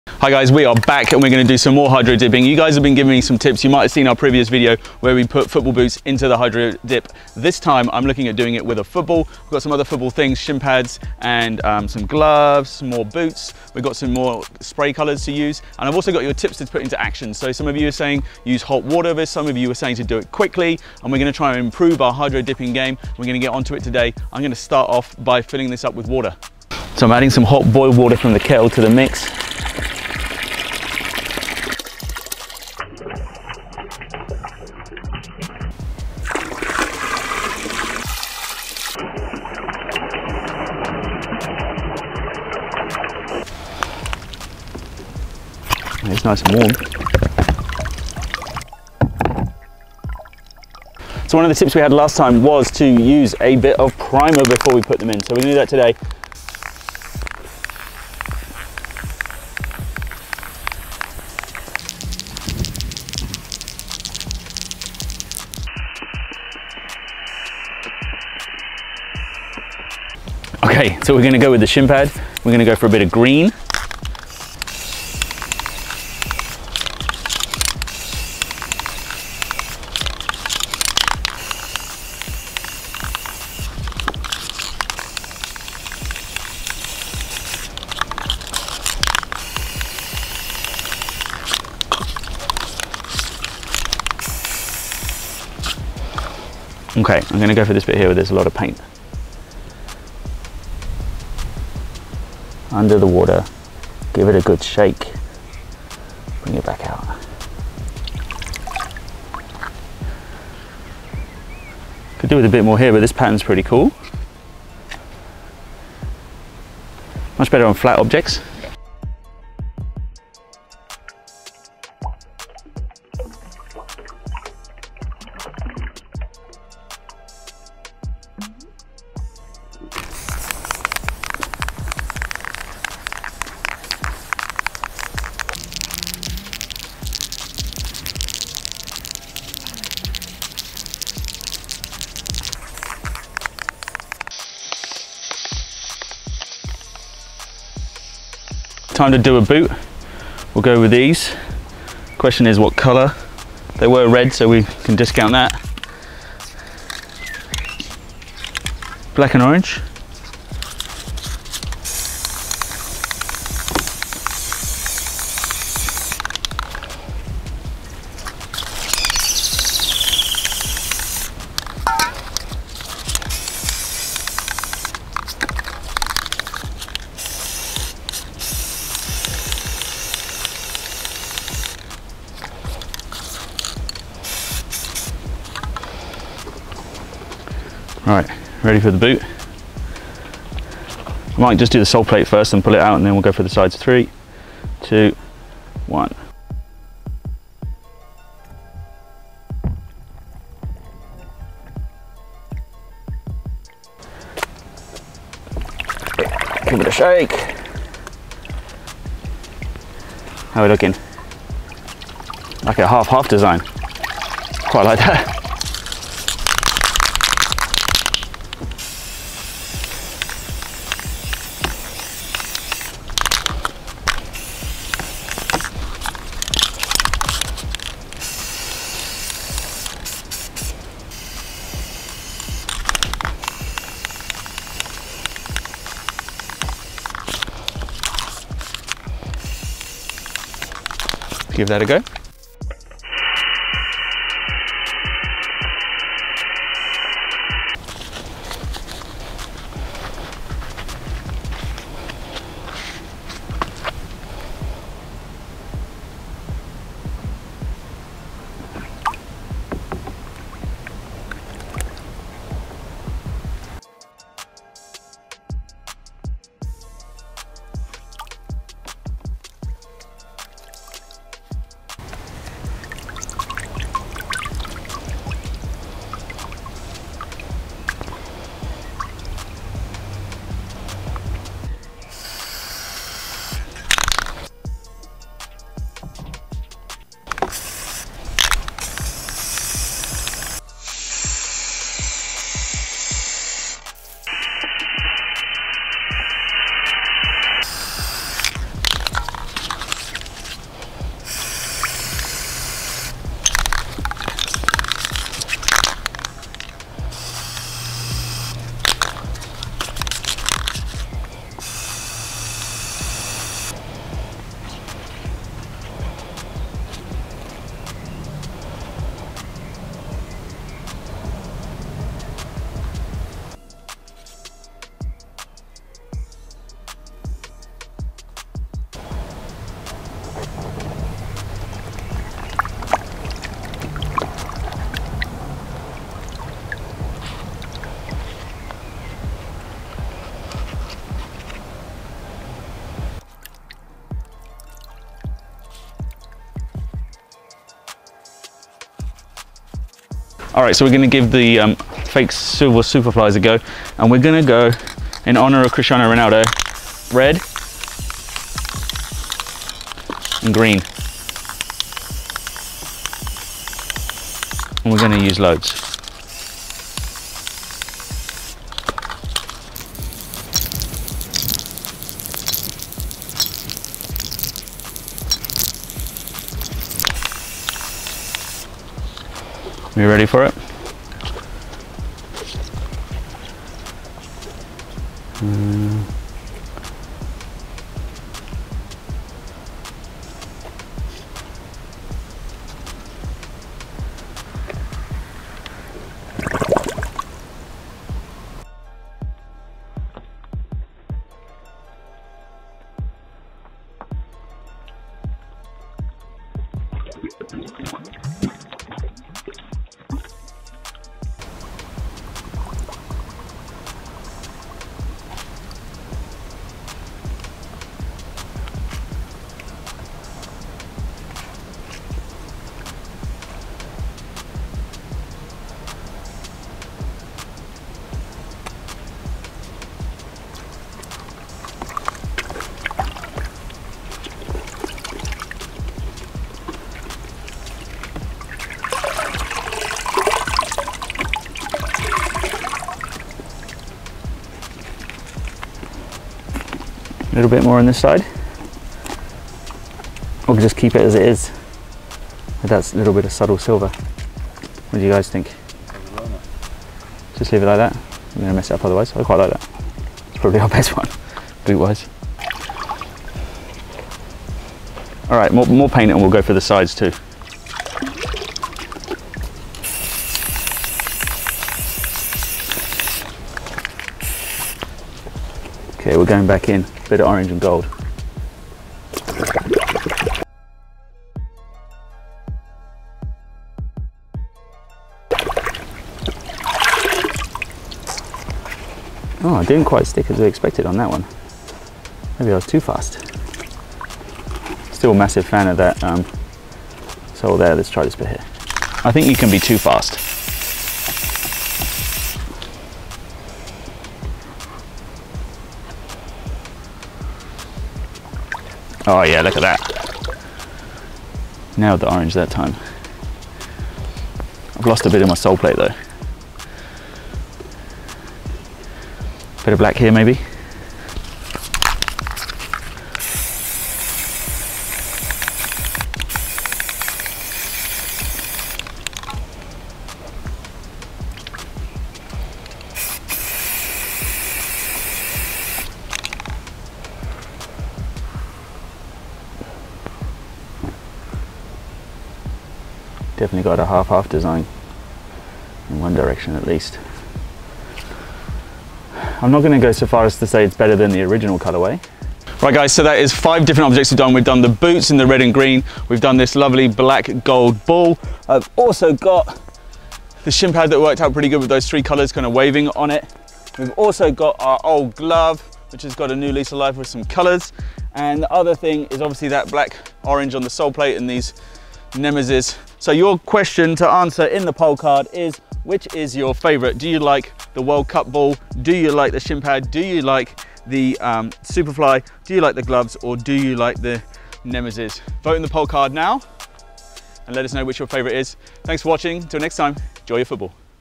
How To Hydro Dip a Football! (Soccer Ball) ⚽️
How do you Hydro Dip a soccer ball? We custom the adidas Telsat and more football gear including shinpads, gloves and boots by hydro dipping them! Put your favourite custom in the comments below!